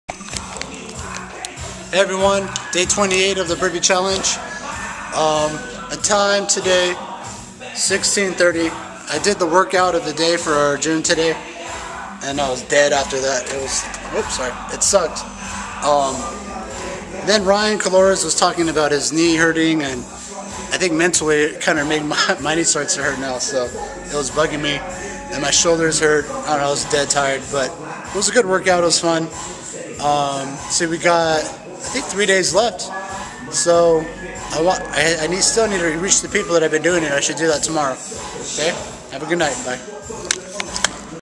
100 Burpee Challenge Day 28
From March 1st until March 31st, complete 100 Burpees each and every day.

Personal accountability, honor, and dedication is integral to the challenge as we promote fitness and a healthy lifestyle.